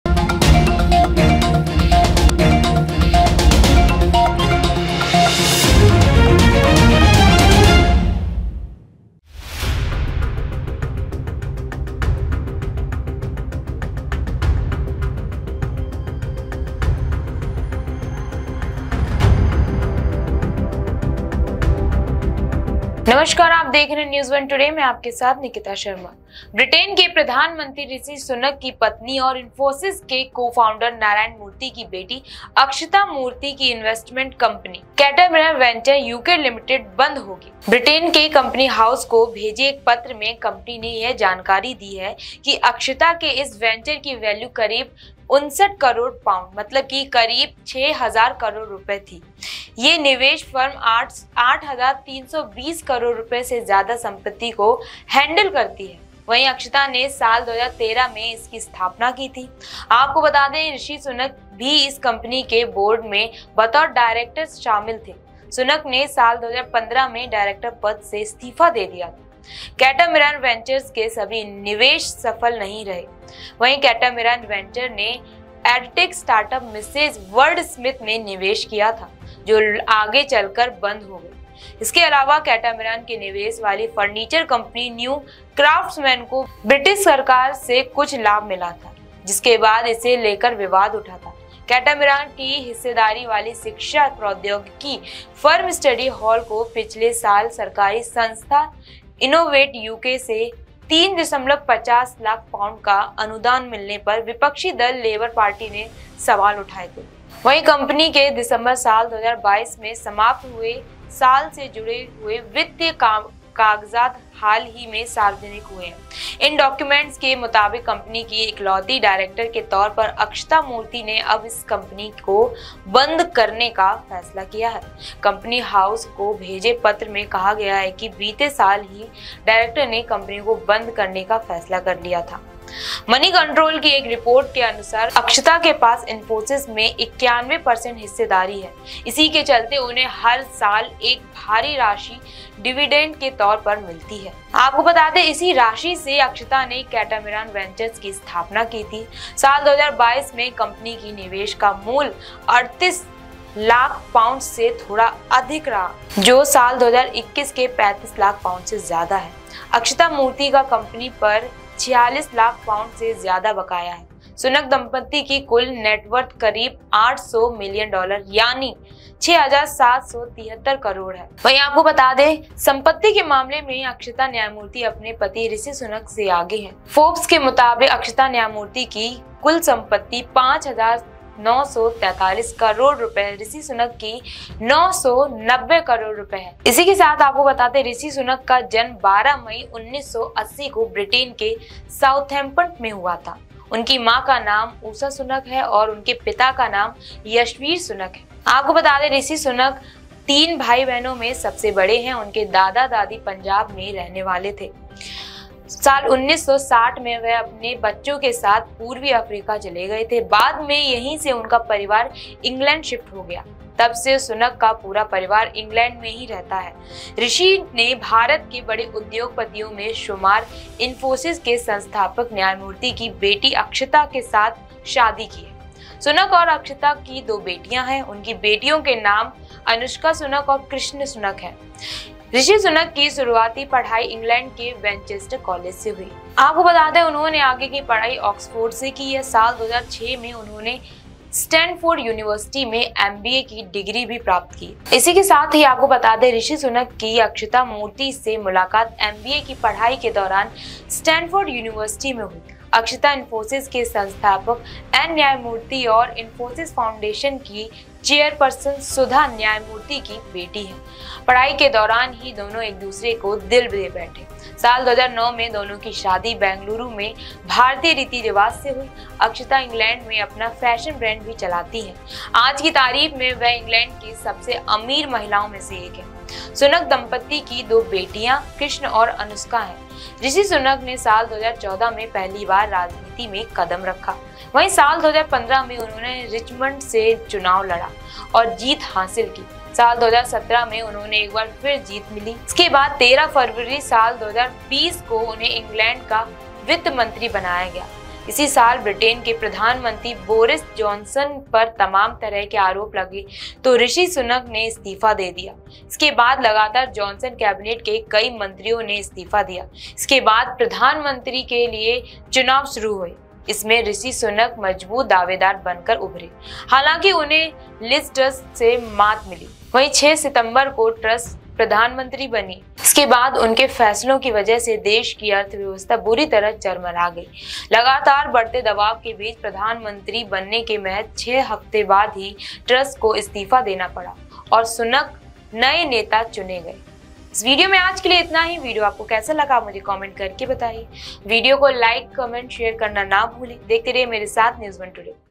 News1 Today | UK PM Rishi Sunak की पत्नी बंद करेंगी अपनी कम्पनी !
Today | UK PM  Rishi Sunak की पत्नी बंद करेंगी अपनी कम्पनी !

Catamaran Ventures UK Limited, the investment company of Akshata Murthy, wife of British Prime Minister Rishi Sunak and daughter of Infosys co-founder Narayana Murthy, will be closed. The company has given this information in a letter sent to Britain's Companies House. The value of this venture of Akshata was around 59 crore pounds (about Rs 6,000 crore). This investment firm handles assets of more than Rs 8320 crore.
ब्रिटेन के प्रधान मंत्री ऋषि सुनक  की पत्नी और इंफोसिस के को-फाउंडर नारायणमूर्ति की बेटी अक्षता मूर्ति  की इनवेस्टमेंट कंपनी कैटामरान वेंचर्स यूके लिमिटेड  बंद होगी. ब्रिटेन के कंपनी हाउस को भेजे एक पत्र में कंपनी ने यह जानकारी दी है. अक्षता के इस वेंचर की वैल्यू करीब 59 करोड़ पाउंड (करीब 6,000 करोड़ रुपये) थी. ये निवेश फर्म 8320 करोड़ रुपये से ज्यादा संपत्ति को हैंडल करती है.